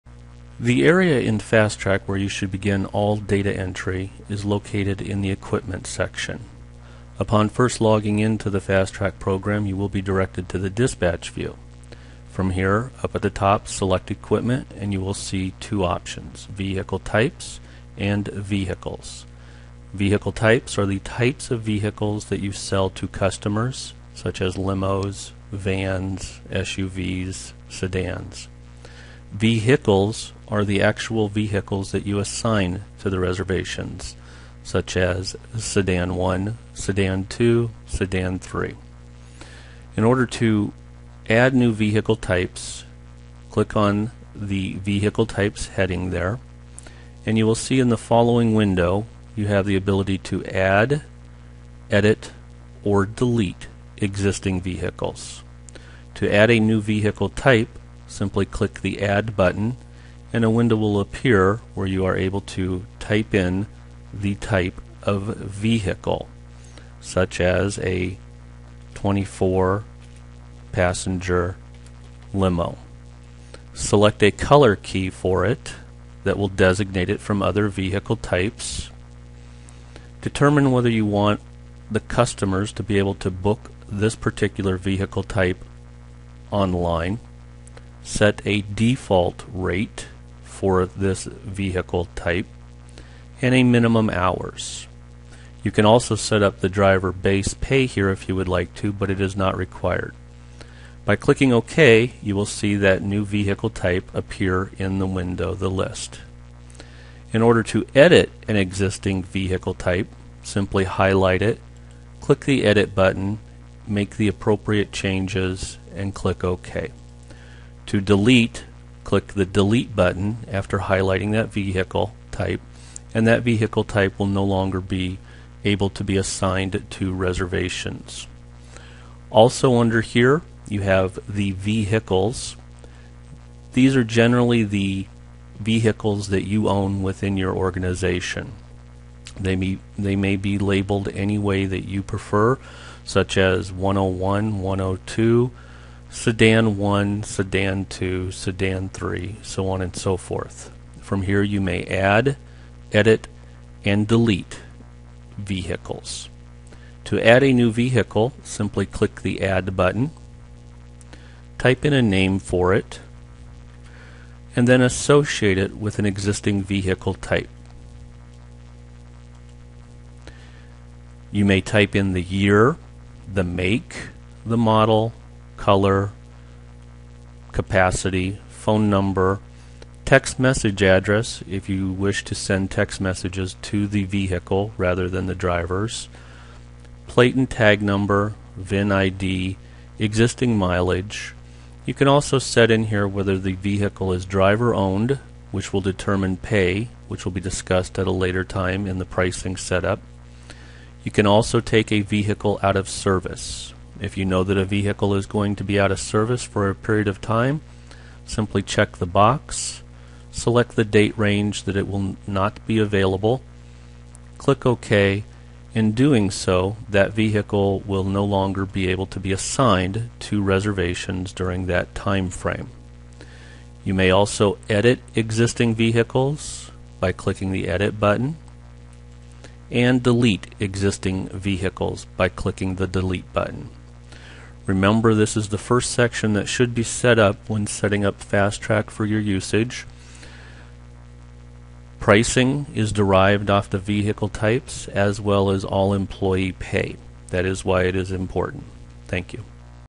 Cloud Express - Mobile - Vehicle Type Setup
Setting up Vehicles & Vehicle Type - by Brian Hood